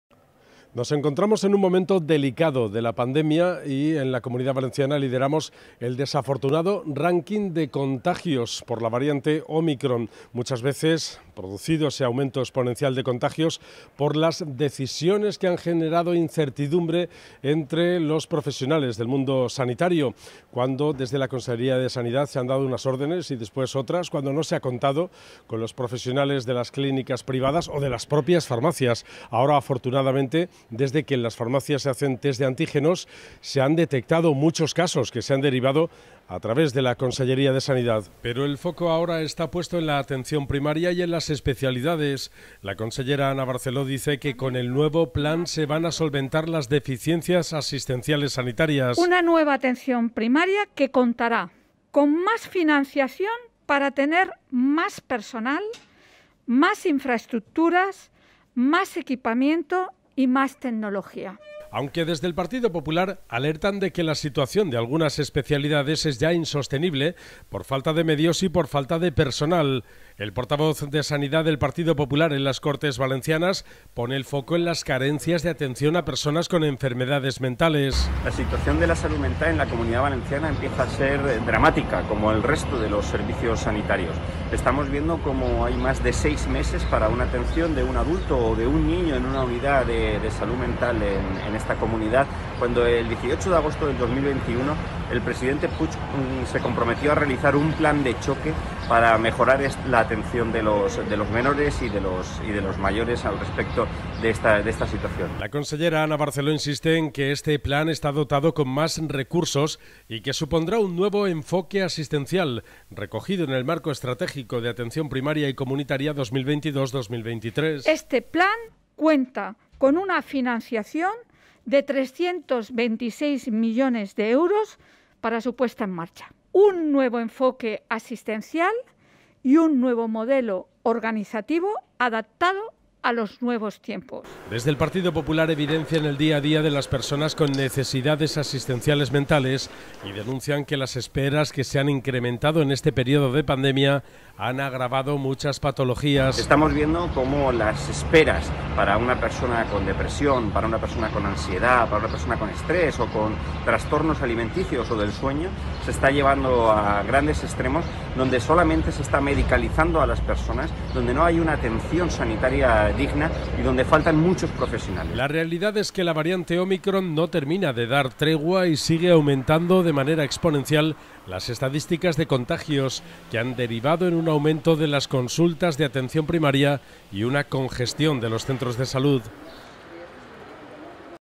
El PP de la Comunitat Valenciana alerta del abandono a las especialidades
Baja la presión en la Atención Primaria gracias a los test en las farmacias y las altas y bajas por internet, pero continúan los retrasos de 6 meses para citas de salud mental.